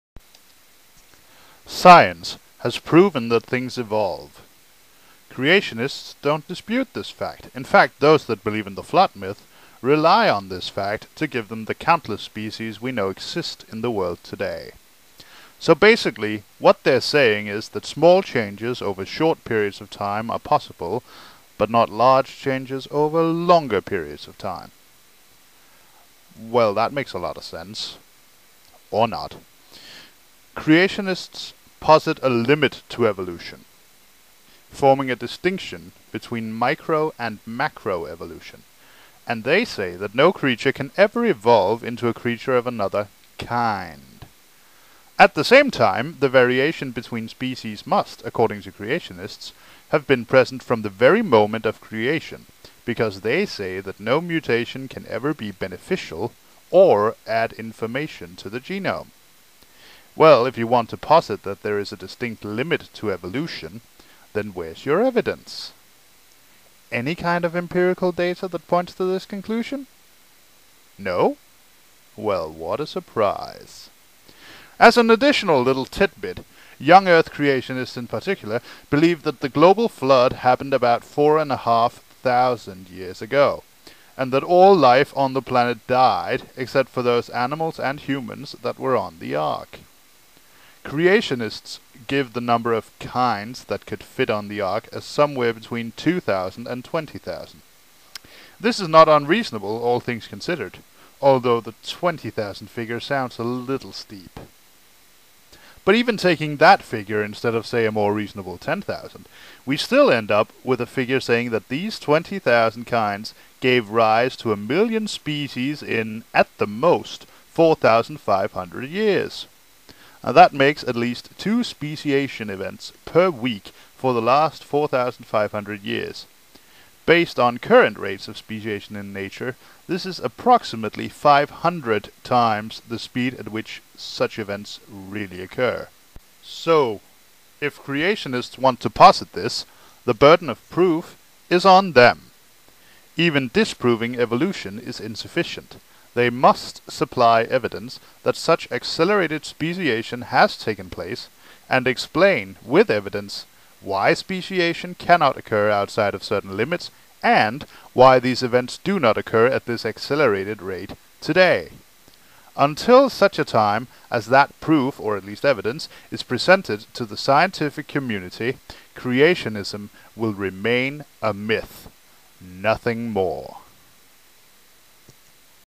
Why the burden of proof lies with creationists
Or, what creationists would have to prove to be taken seriously.

I made a mistake in the very first sentence of this video. It should of course be "living things", not simply "things".
Oh well, I hope the point comes across clearly anyway.